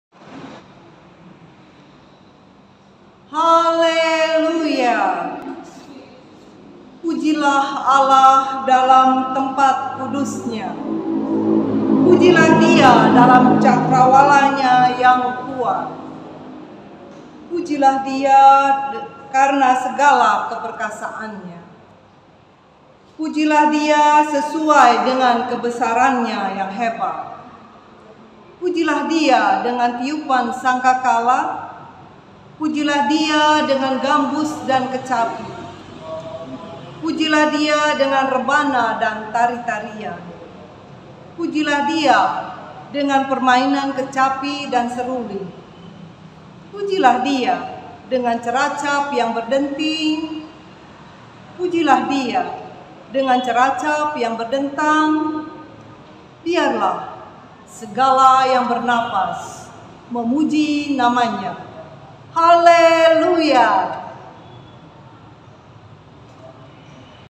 Kategori: Baca Puisi - PKLU GPIB Gibeon -2, JAKSEL
Pagelaran Seni PKLU GPIB dalam rangkah HUT Ke-11 PKLU GPIB 2021
Kelompok Kategori: Baca Puisi

Video yang ditampilkan adalah untuk memperoleh juara favorit berdasarkan jumlah "like" / jempol yang terbanyak.

Video-video ini adalah yang telah melewati seleksi seturut dengan kategorinya masing-masing, yaitu dalam hal:
Durasi Waktu: maksimum 2  Menit
Jumlah peserta maksimum  2 orang